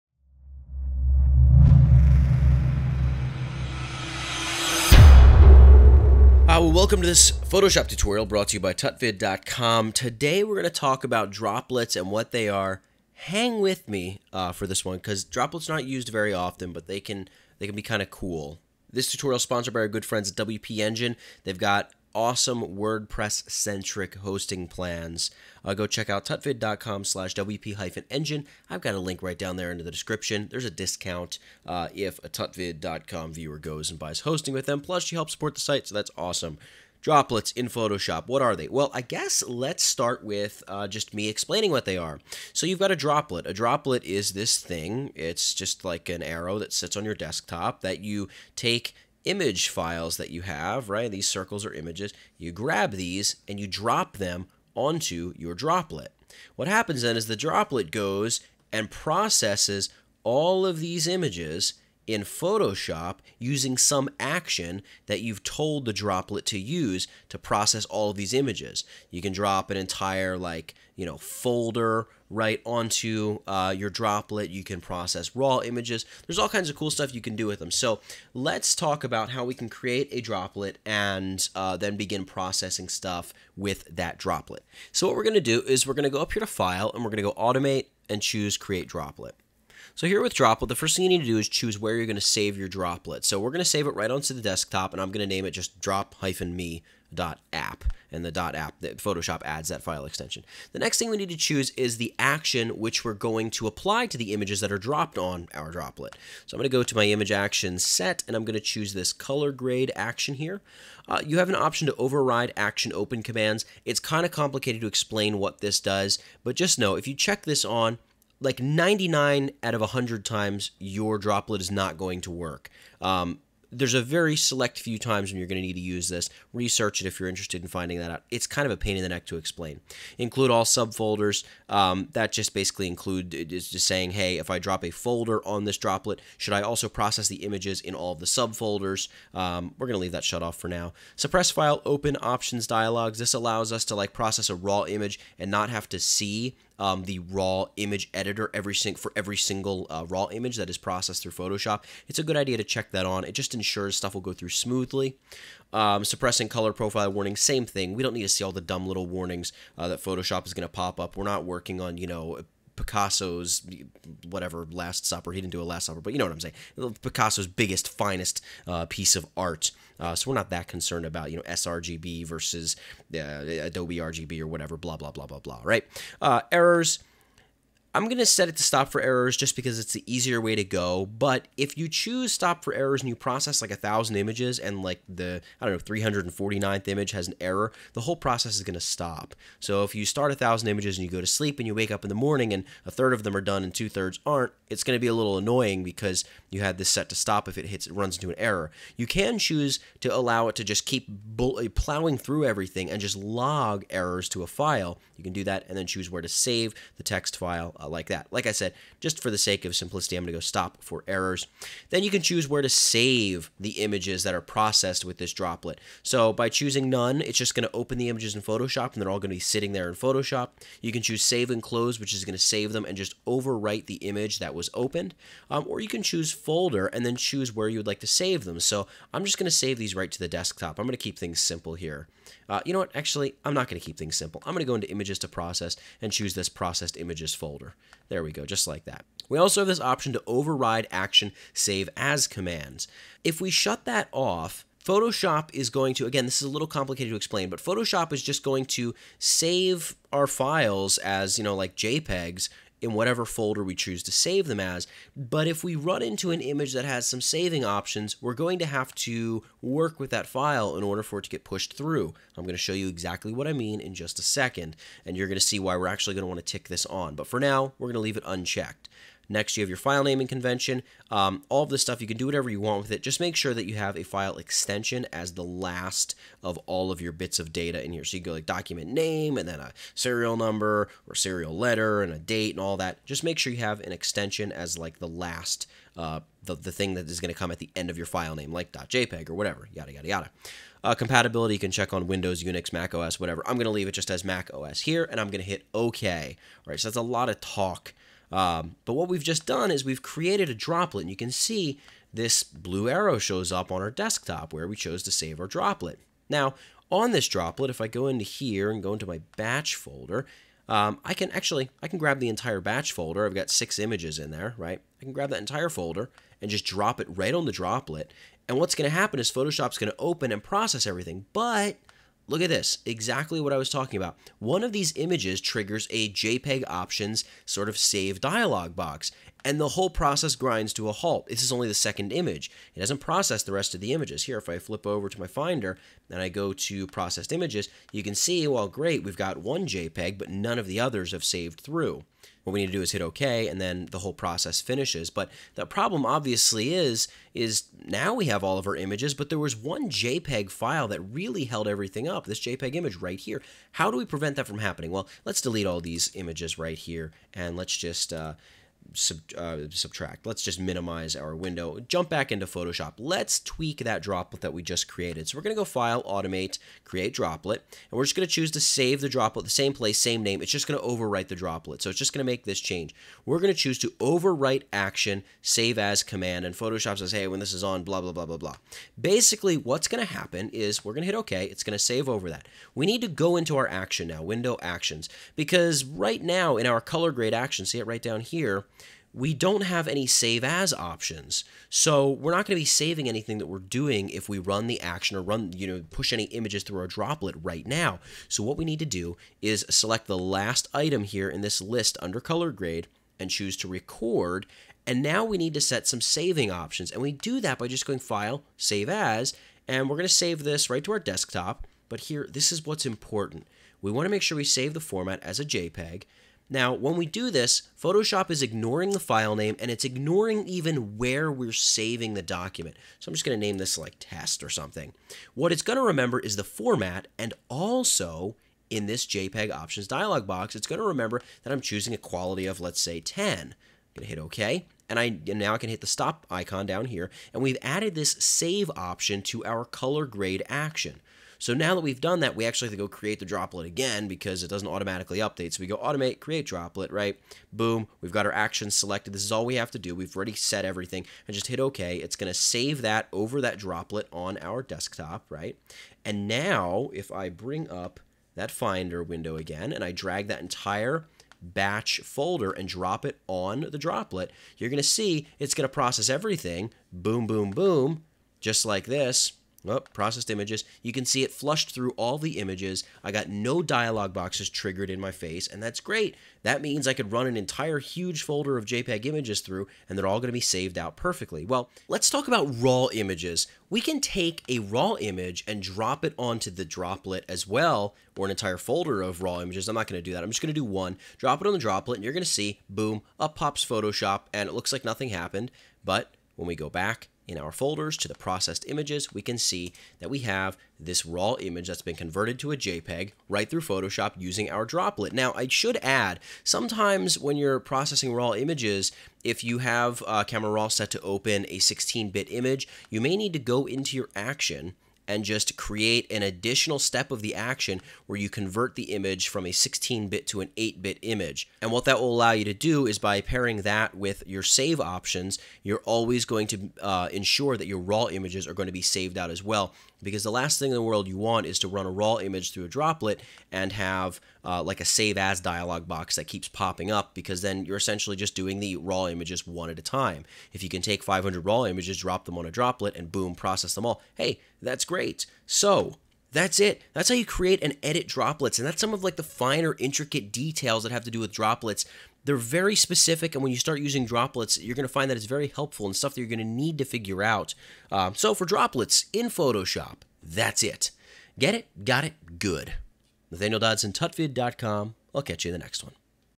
What, Why, & How?? -Photoshop CC Droplets
Have you ever used a droplet? Droplets were fairly popular back in 2009 or so, but they still have some good use. You can use them to quickly batch process images or entire folders of images even without Photoshop open! Droplets are easy to share as well if you have a team all working on the same project. Check out this tutorial and start using droplets for yourself today! Image used in this tutorial

WP Engine is the sponsor of this video! and get the exclusive discount for tutvid.com viewers!